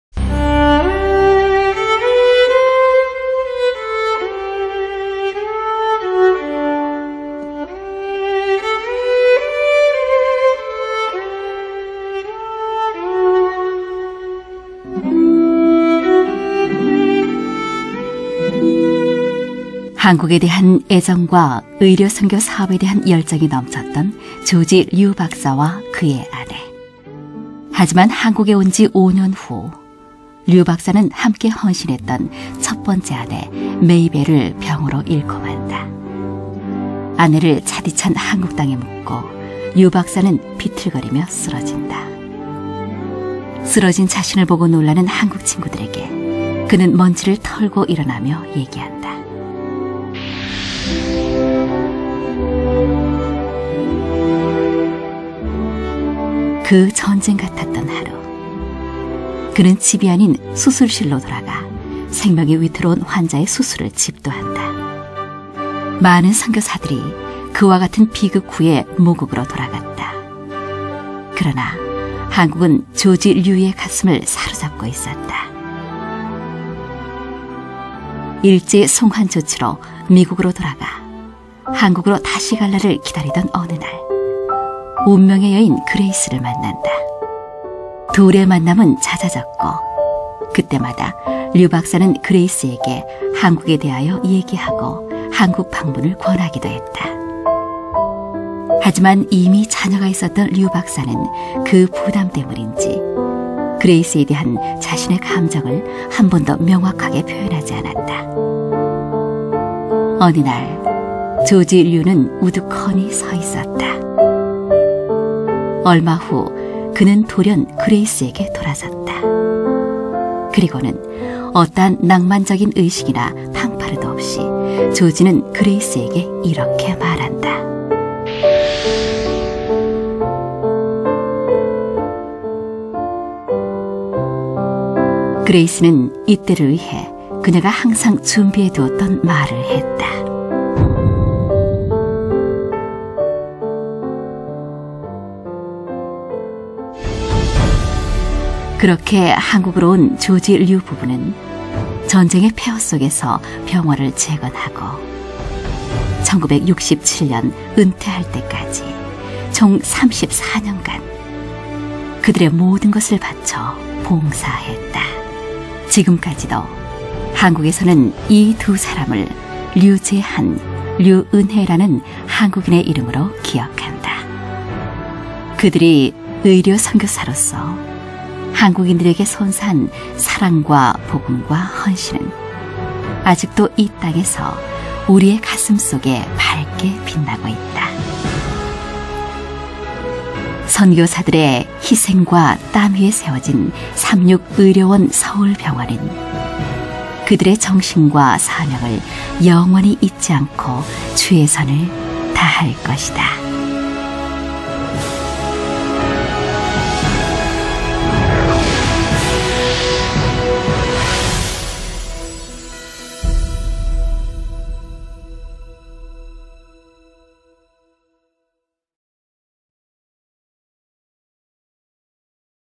삼육서울병원의 아버지, 류제한 박사 (Rue, Georgy Henry) 일대기
[삼육서울병원 역사 이야기]

류제한 박사는 이승만 대통령의 주치의이자 삼육서울병원(구 서울위생병원)의 초석을 놓은 것으로 유명합니다.
선교를 목적으로 고향을 떠나 먼 타지에서 의료 봉사를 했던 그는, 은퇴 후에도 6번이나 한국을 찾는 등 마지막까지 한국에 대해 변함없는 사랑을 보여주었어요.
류제한 박사의 일대기를 애니메이션으로 만나보세요!

더 자세한 이야기는 삼육서울병원 포스트에서 확인하세요!